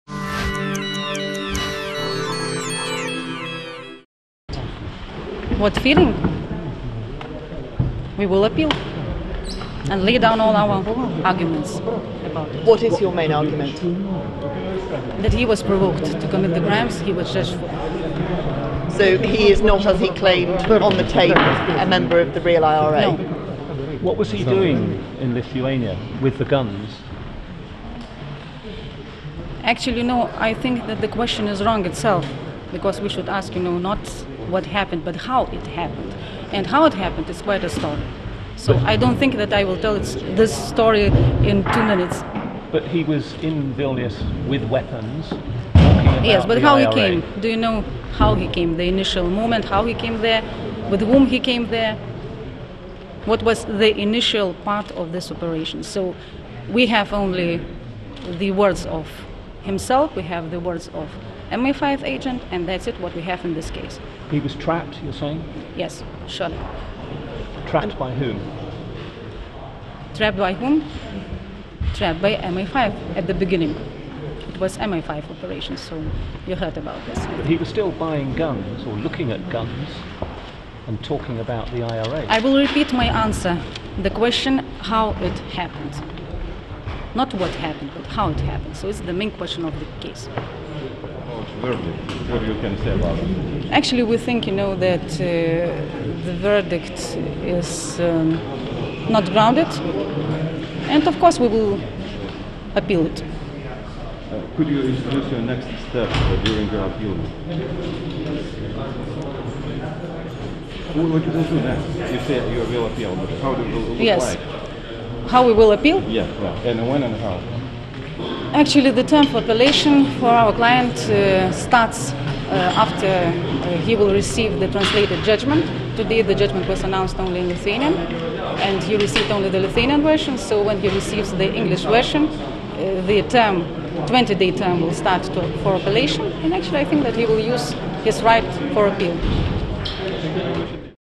Lawyer's Ingrida Botyrienė interview after court verdict in Michael Campbell case
On friday Michael Campbell was sentenced to 12 years in prison for attempting to buy weapons in Lithuania. His lawyer I.Botyrienė said she is going to appeal for this verdict.